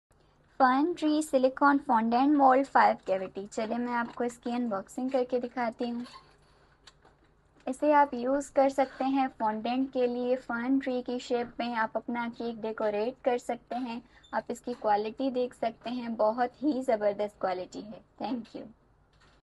Bake House | Fern Tree Silicone Fondant Mold 5 Cavity
The Fern Tree Silicone Fondant Mold 5 Cavity is perfect for creating detailed fern tree designs for cakes, cupcakes, and other treats. With five cavities, this mold allows you to easily shape intricate fern tree patterns using fondant, gum paste, or chocolate. Made from flexible, high-quality silicone, it ensures smooth release and durability, making it a great tool for adding natural, botanical details to your baked creations.